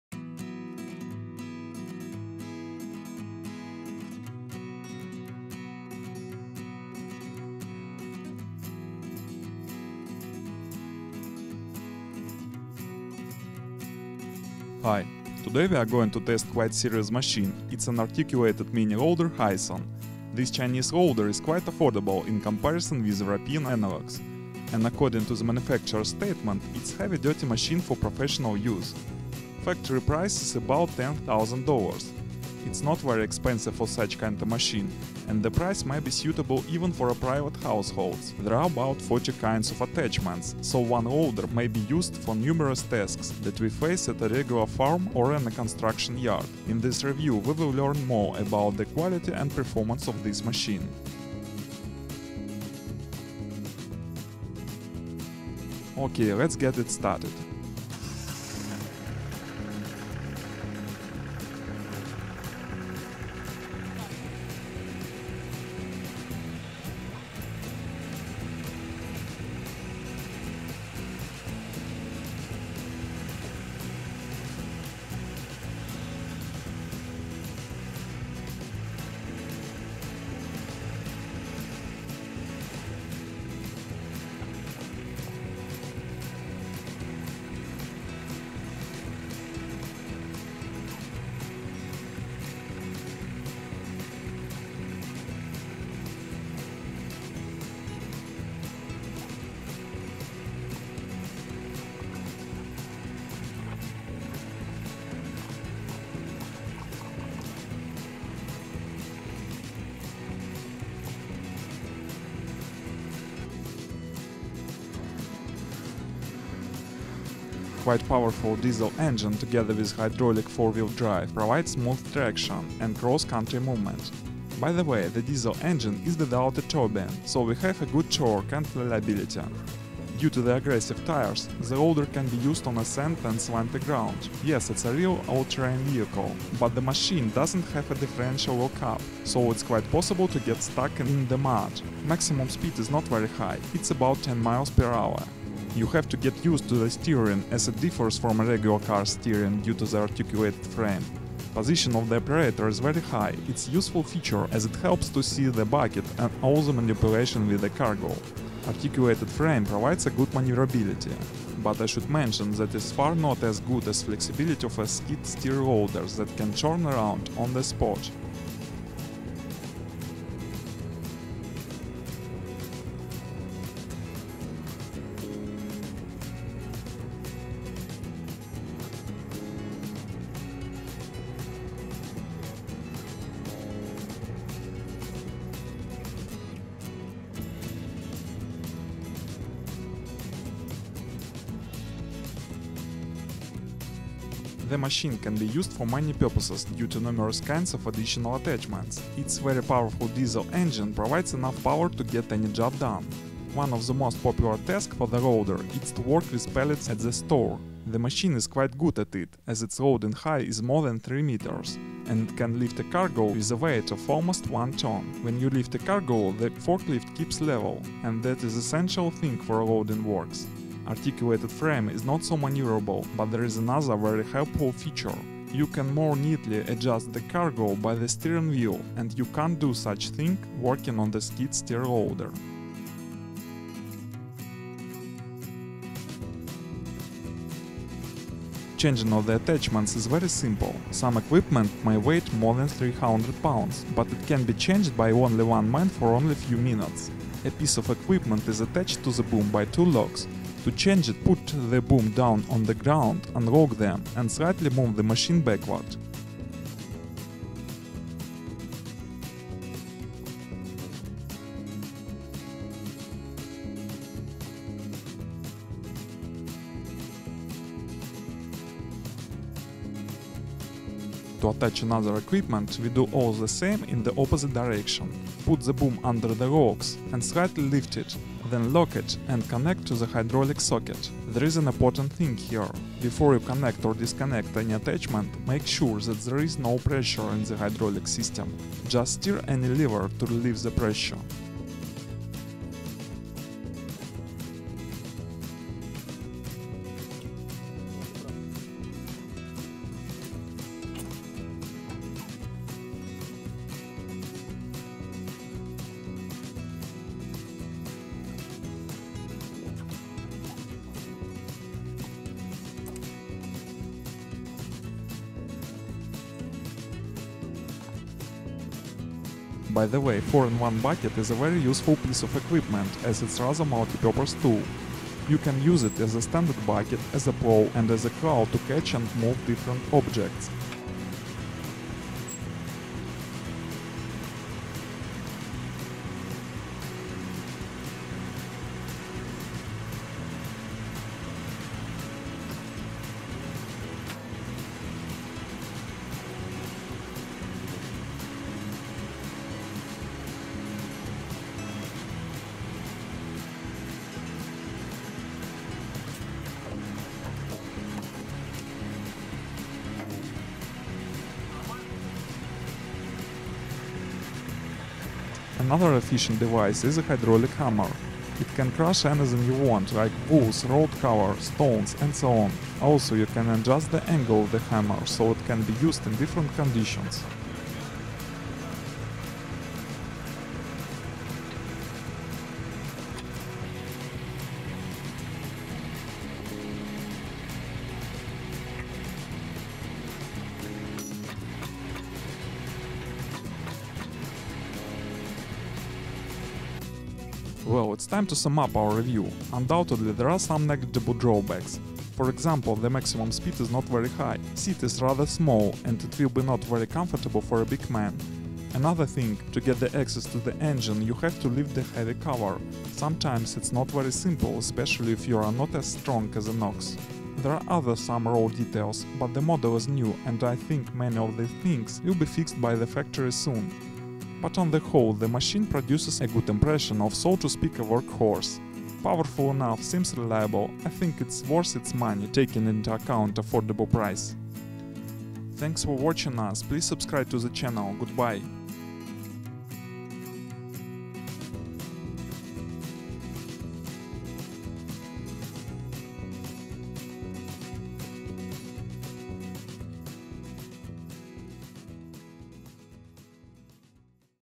Cheap and powerful mini loader Hysoon HD10L, full review
Type "hysoon loader" in search.

Today we are going to test quite serious machine – it’s an articulated mini loader Hysoon. This Chinese loader is quite affordable in comparison with European  analogs, and according to the manufacturer’s statement it’s a heavy duty machine for professional use. Factory price is about 10 000 dollars, it’s not very expensive for such kind a machine and the price may be suitable even for a private households. There are about 40 kinds of attachments , so one loader may be used for numerous tasks, that we face at a regular farm or on a construction yard. In this review we will learn more about the quality and performance of this machine.